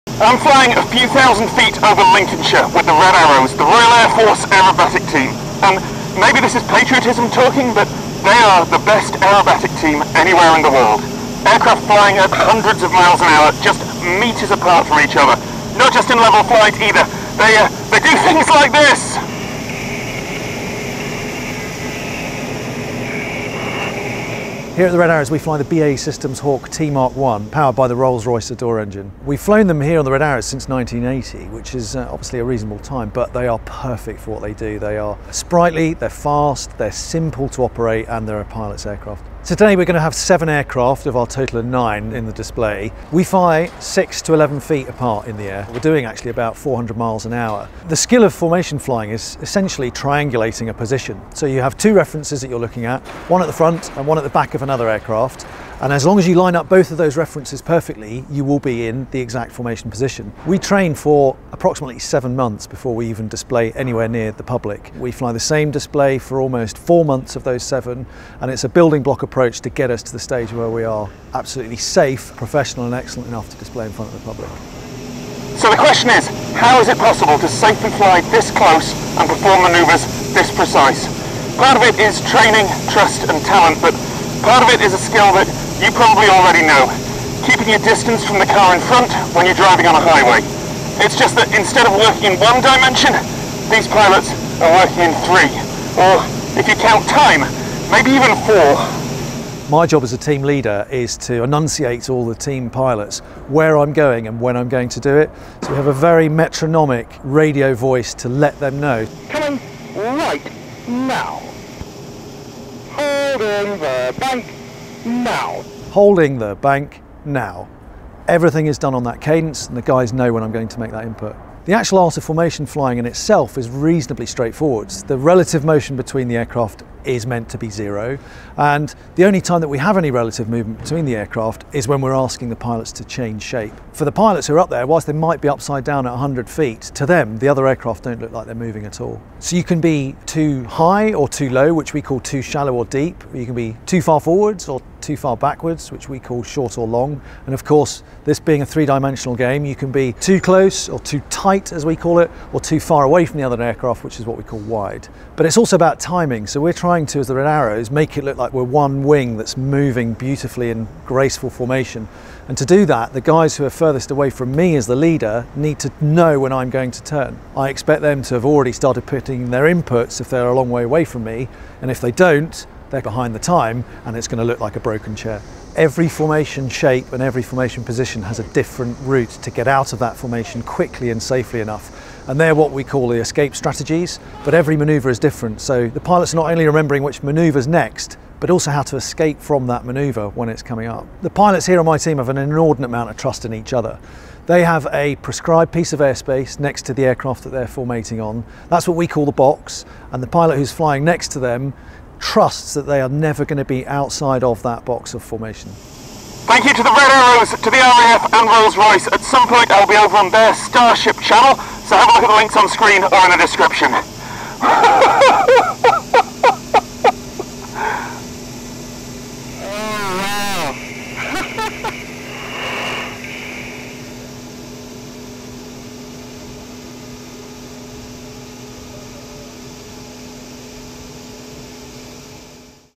How formation flying works
Thanks to the RAF, Rolls-Royce, and the Red Arrows! Go visit their Starrship project, it's the reason I got to do this

The Red Arrows are the Royal Air Force's aerobatic display team - the best in the world. They fly Hawk T1 jets, powering through the sky at hundreds of miles per hour, pulling high-G maneuvers with just a few metres between their wingtips. Here's how they do it: and part of it's a skill that you probably already know.